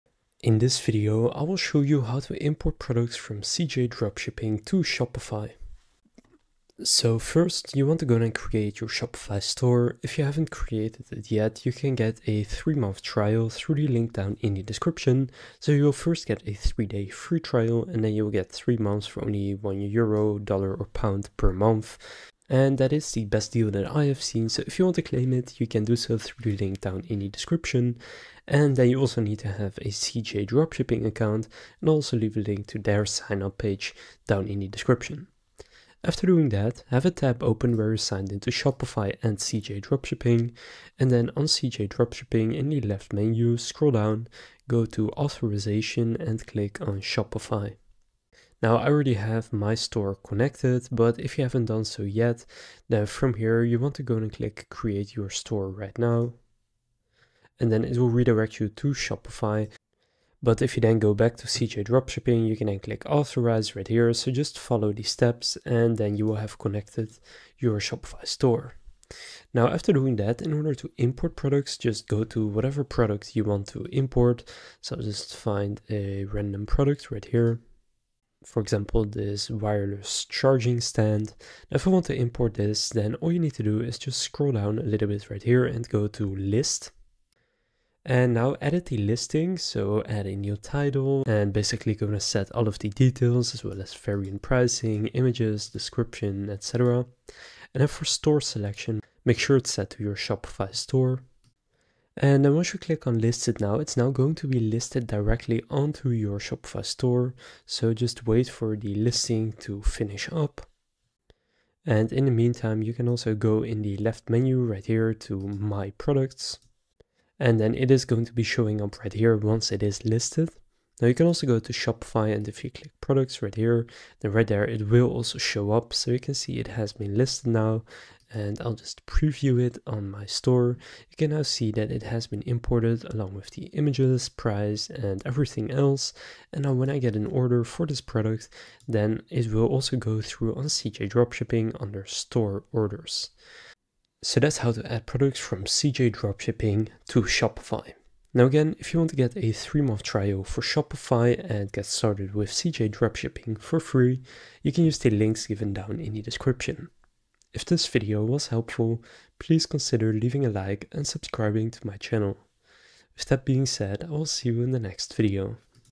How To Add Products To Shopify From CJ Dropshipping (2026)
In this video I'll show you How To Add Products To Shopify From CJ Dropshipping.